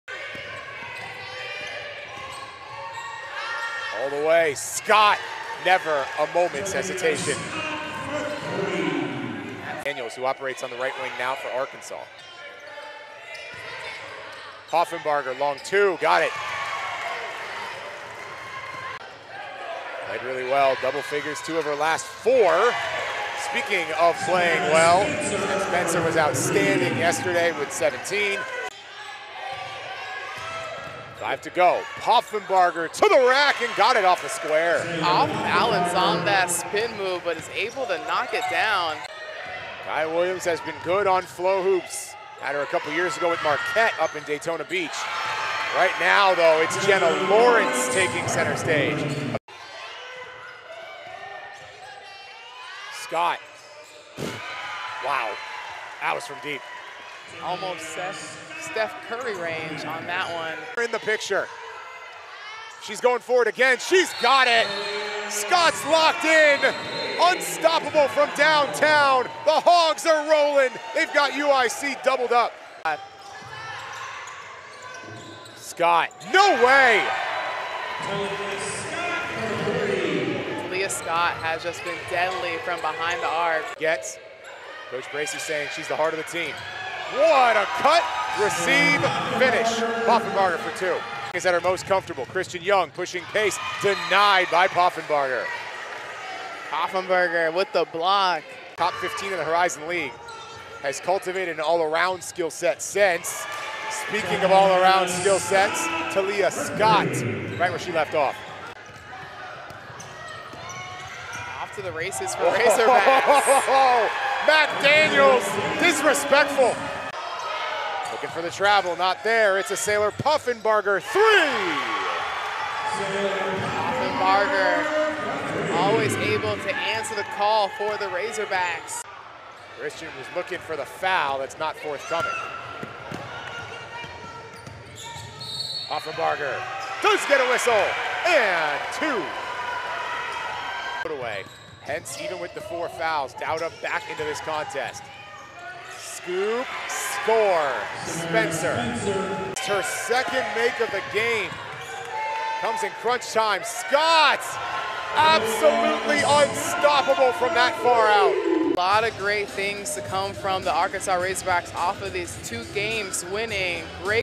Highlights: Hogs Defeat UIC | RAZORBACK BASKETBALL
The Arkansas Razorbacks defeated UIC in their second game of the West Palm Beach Classic.